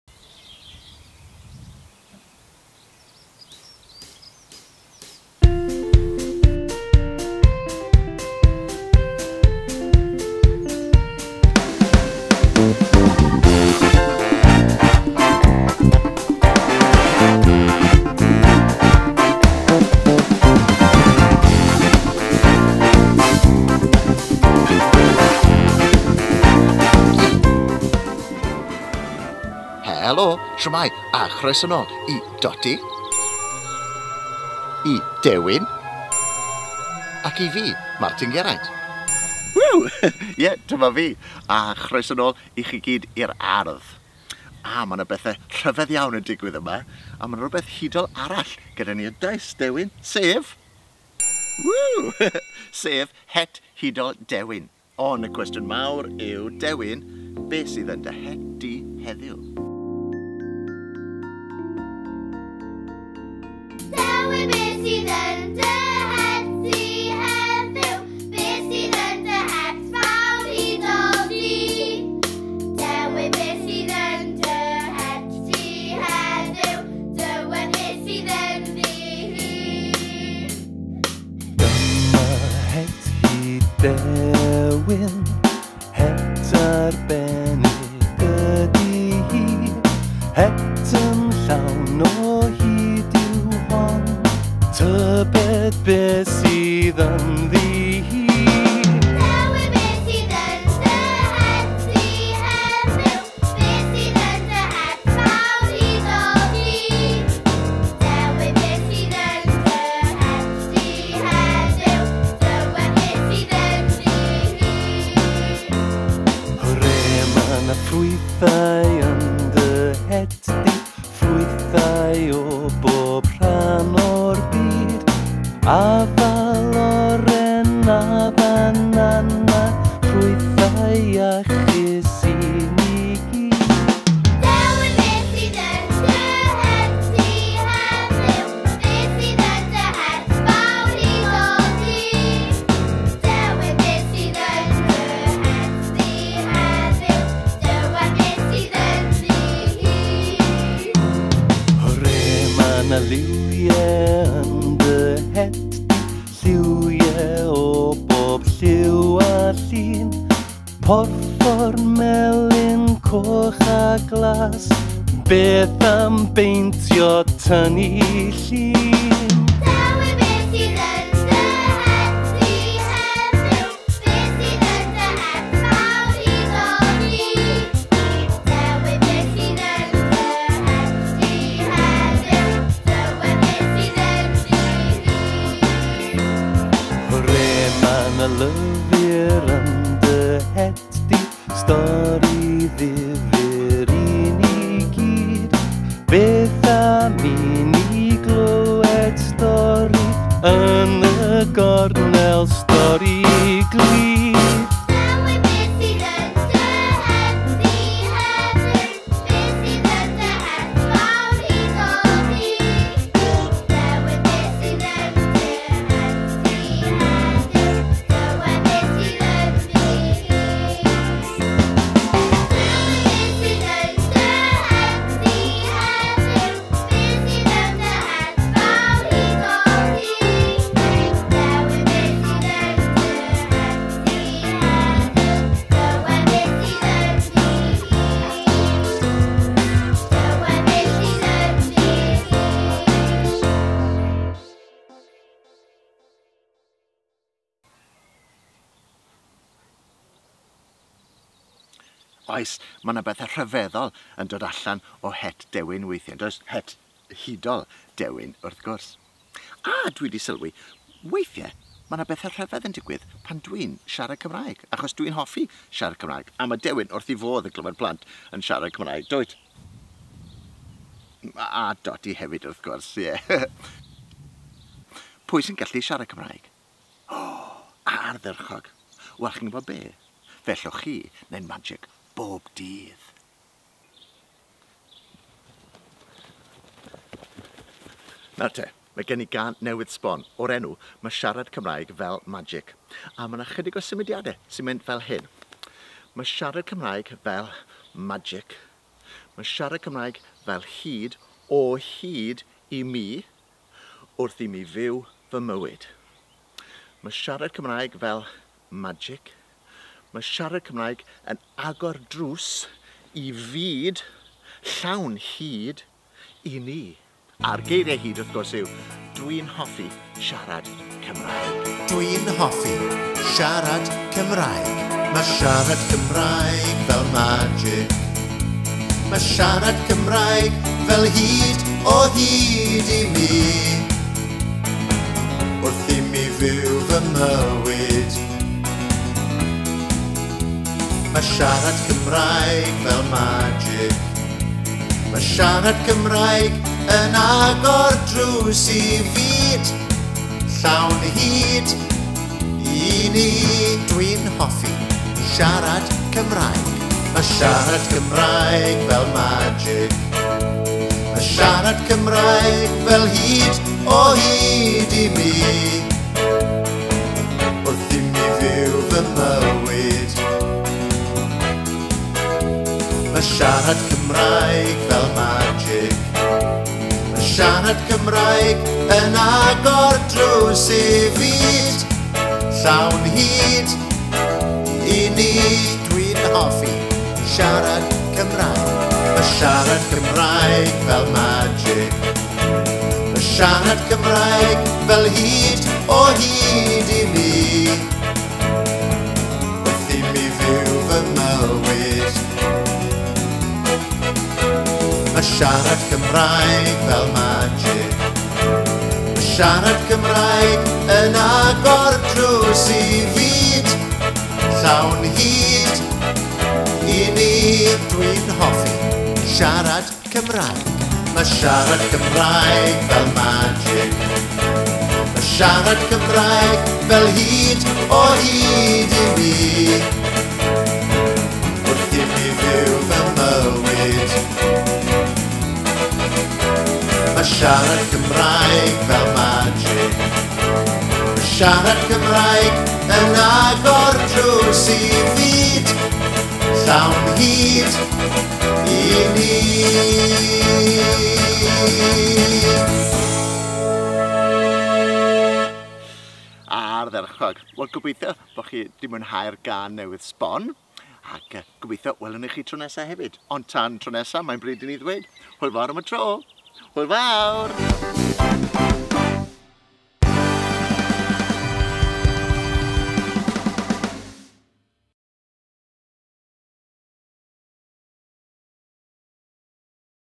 Dewin, Doti a'r Het Hudol - Rhan 3
Mae Gŵyl Dewin a Doti 2020 yn parhau! Dyma drydedd rhan Sioe ‘Dewin, Doti a’r Het Hudol’ gyda Martyn Geraint, Dewin a Doti - mwynhewch! 🤩

Gŵyl Dewin a Doti 2020 continues! Here's the third part of the show, ‘Dewin, Doti a’r Het Hudol’ with Martyn Geraint, Dewin a Doti - enjoy! 🤩